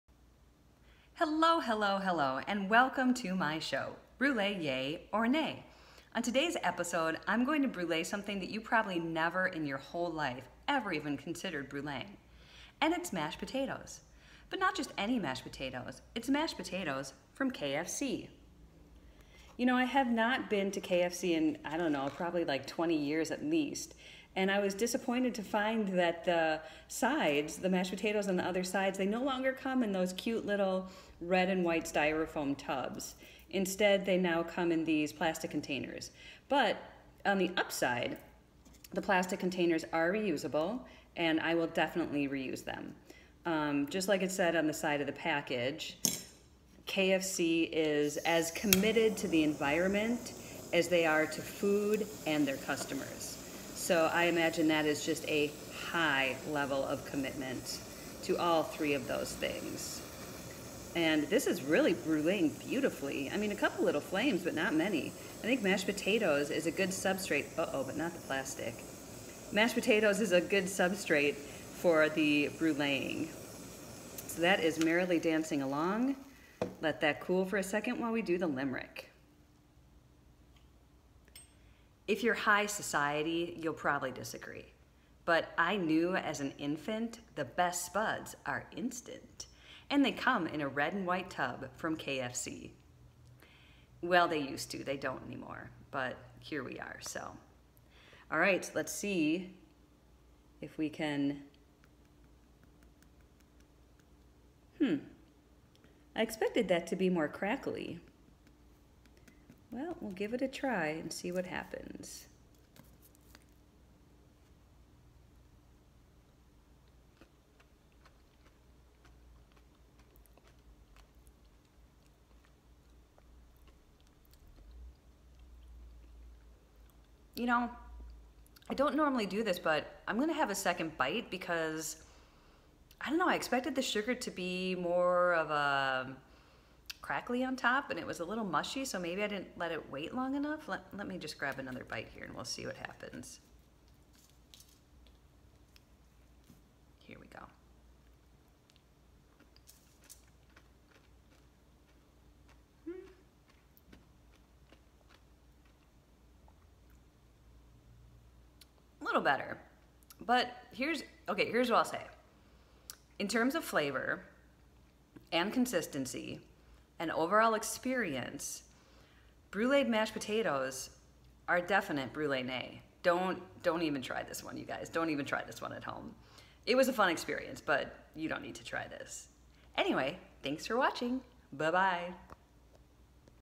Brûlée Yay or Nay - mashed pots
A woman brûlées mashed potatoes from KFC and you won't believe what happens!

S01E18